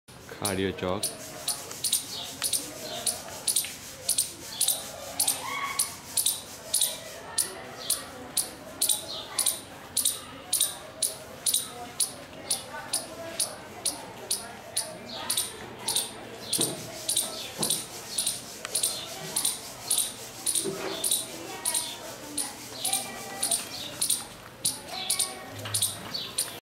Khadiyo chock chanasma
It was really nice and peaceful afternoon. .  I love my village and my chock, jay khadiya chock.